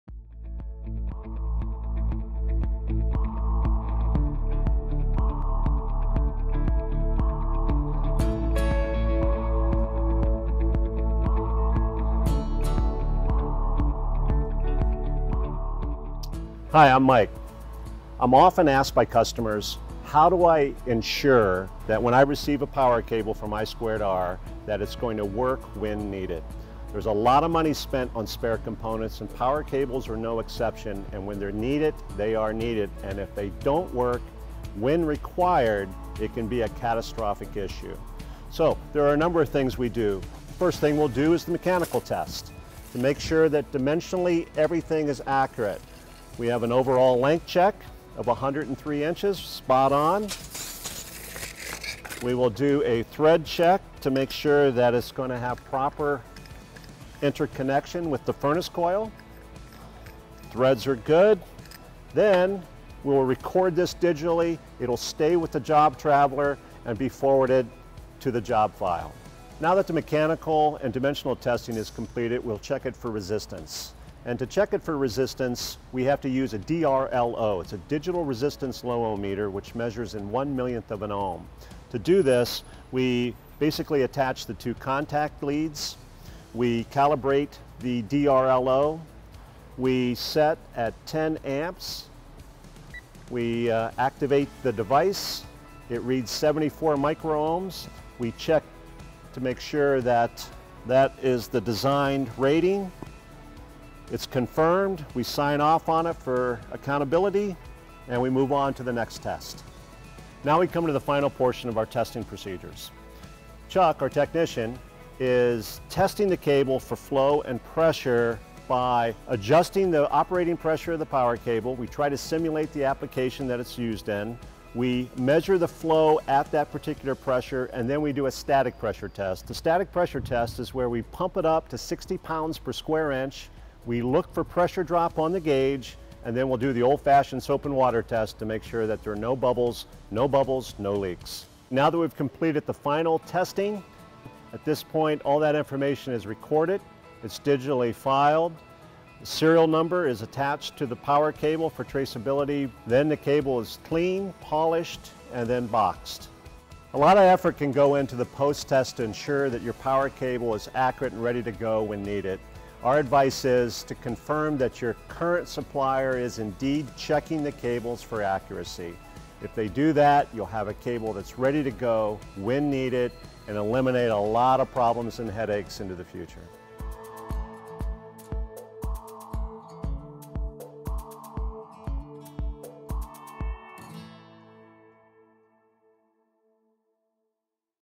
How Will I Know if my New Spares Work?
Stakes are high. A furnace is down. Your spare induction power cable is taken from the tool crib. What if it is too short, the wrong size, or poorly constructed? Now a problem becomes a very big, expensive issue. Customers often ask us how we test a cable so it isn't one more thing to worry about.